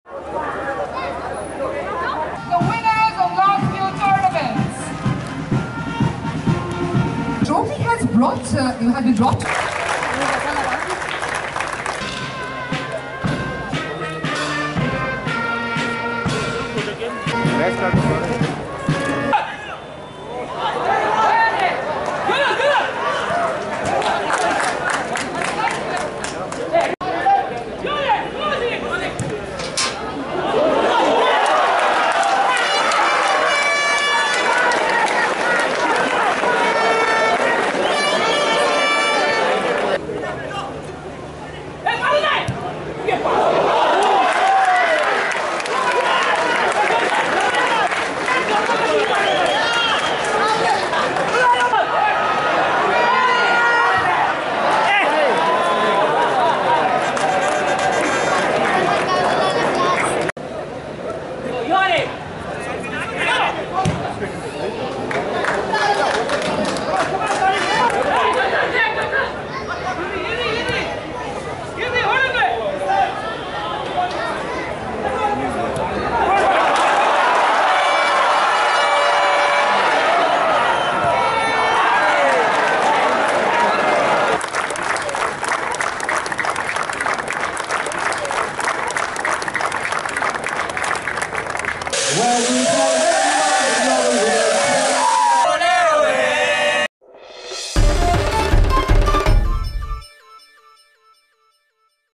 PUNE FC WIN BANDODKAR GOLD TROPHY
Pune FC thrash Dempo Sports Club 4-1 to win Bandodkar Gold Trophy U-21 football tournament at Duler Stadium in Mapusa